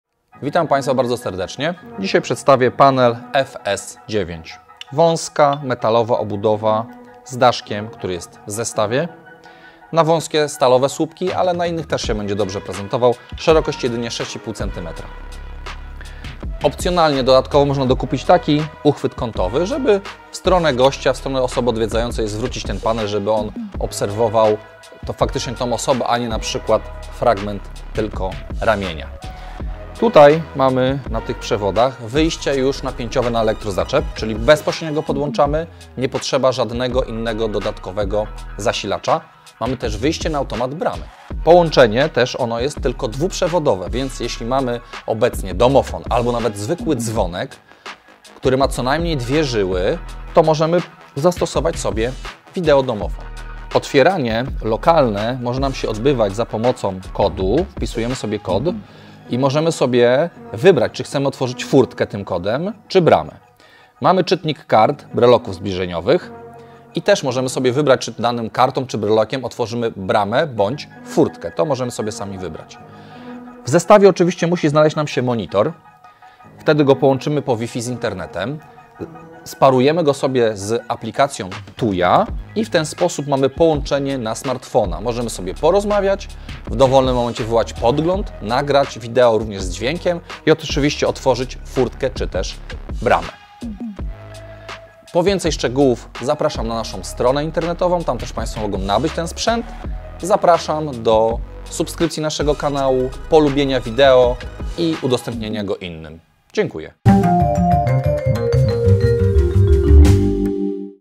Stylowy, prosty w konfiguracji cyfrowy wideodomofon z aplikacją Tuya Smart F-S9-2W
Cała seria domofonów serii F oparta jest na komunikacji dwu przewodowej. Oznacza to, że do poprawnego działania systemu wymagane są jedynie dwa przewody dzięki wykorzystaniu transmisji cyfrowej. Seria F charakteryzuje się płynnym działaniem, nie ma problemów z urywającymi się połączeniami i niedosłyszeniem rozmówcy. Posiadając domofon lub zwykły dzwonek możesz w łatwy sposób wymienić klasyczne urządzenie na nowoczesny wideodomofon z aplikacją (przy zastosowaniu monitora wewnętrznego z serii F). Do niego można podłączyć automat bramy i furtkę bez konieczności stosowania dodatkowego zasilacza, bo panel zasili zaczep blokujący furtkę.